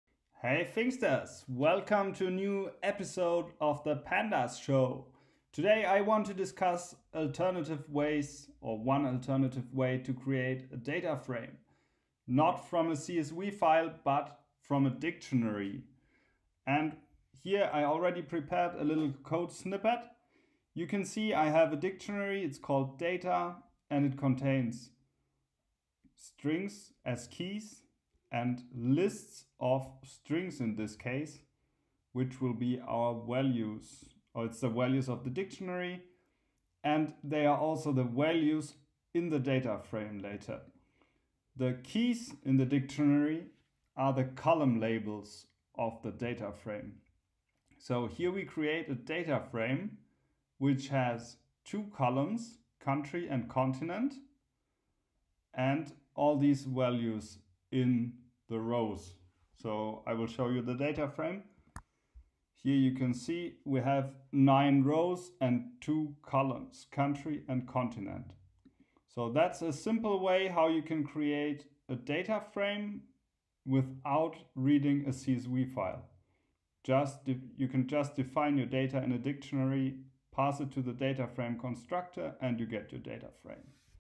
How to Create a DataFrame in Pandas | From a Dictionary
There are many ways to creating new dataframes in pandas, one of them is using a dictionary of lists. In this video I explain you how.

Do you want to thrive as a self-employed Python freelancer controlling your own time, income, and work schedule?